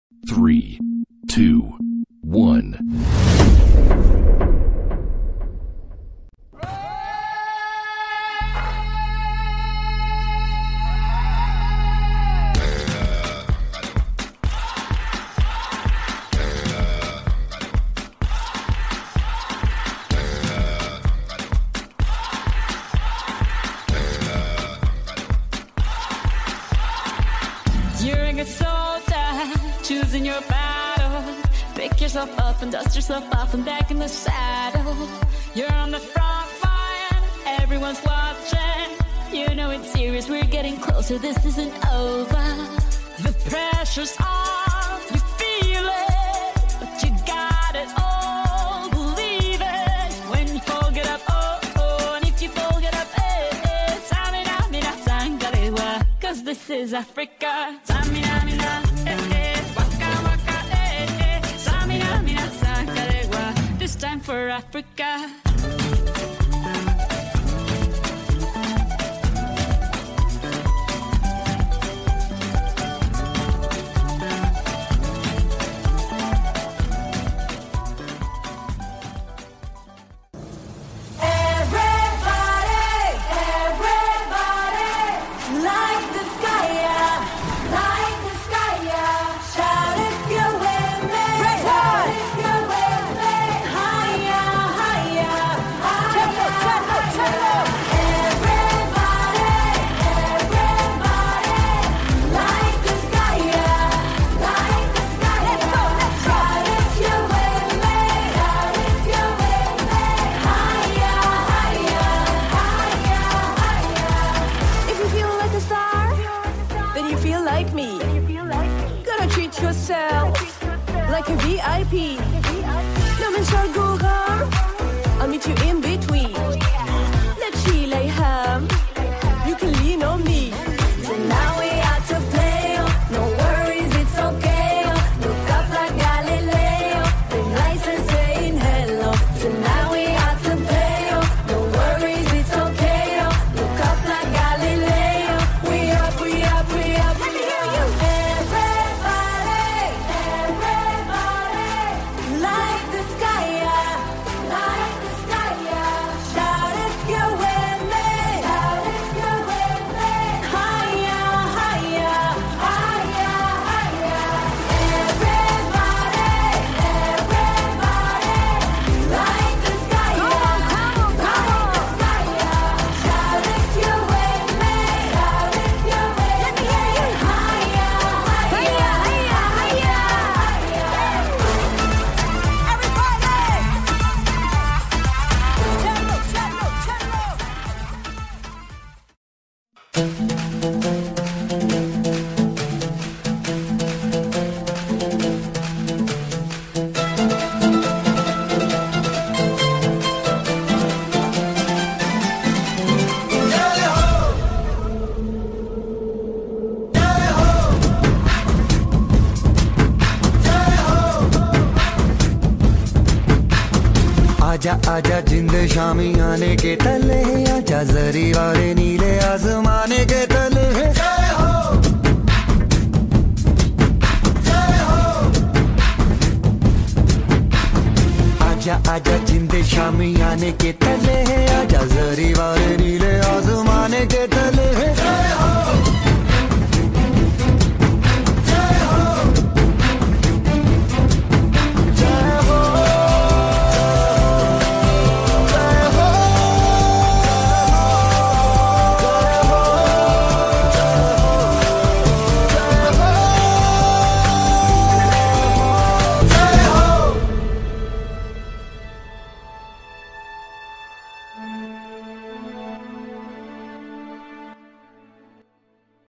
#Cheer Dance Performance (MLZS ARA) on the occasion of annual sports day Chore by Mr Manish & Monty
A Cheer Dance Performance in Mount litera Zee school Bampali Ara on the occasion of annual sports day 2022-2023